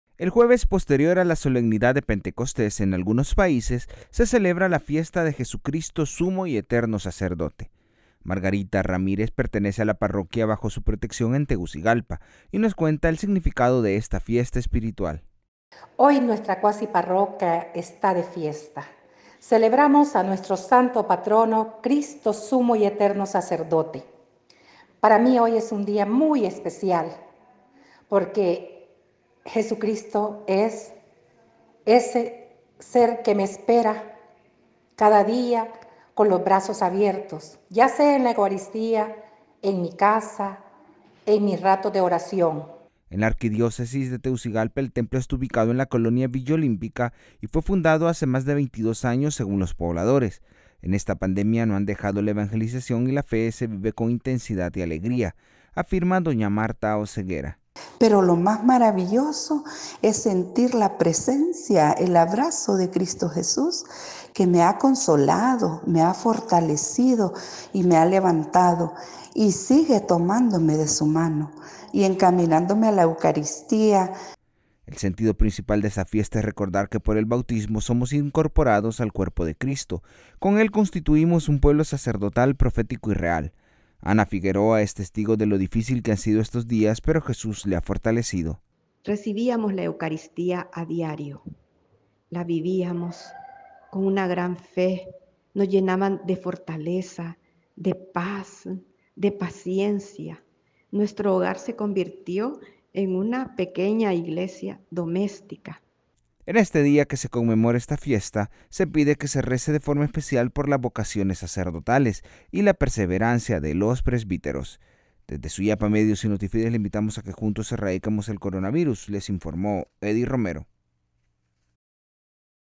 Jesucristo Sumo y Eterno Sacerdote
Celebramos la fiesta de Jesucristo Sumo y Eterno Sacerdote, una conmemoración que nos recuerda la entrega por amor que ha hecho el Señor.